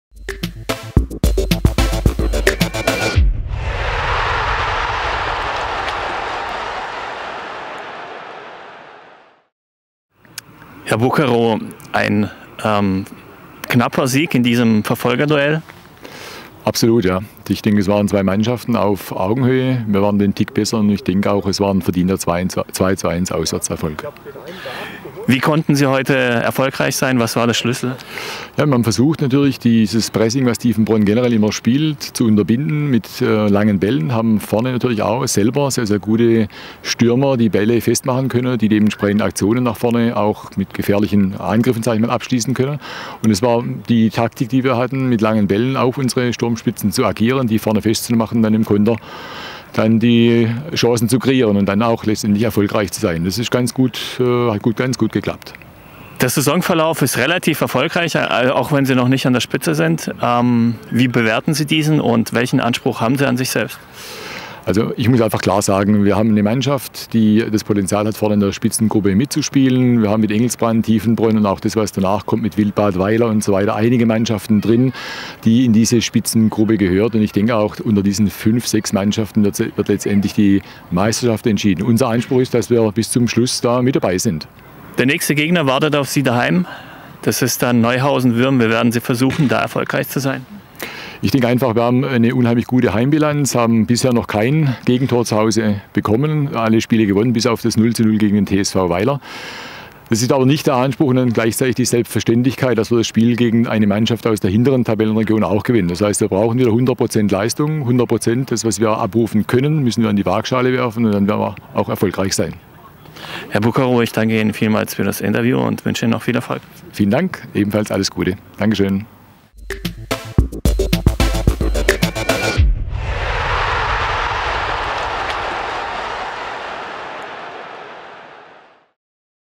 Marcus Bouquerot - FV Langenalb - zum Spiel vs. FV Tiefenbronn im FuPa.tv-Interview am 30.10.2016
Marcus Bouquerot, Trainer des FV Langenalb, im FuPa.tv-Interview zur Begegnung FV Tiefenbronn - FV Langenalb am 11. Spieltag der Kreisklasse A2 Pforzheim 2016/17.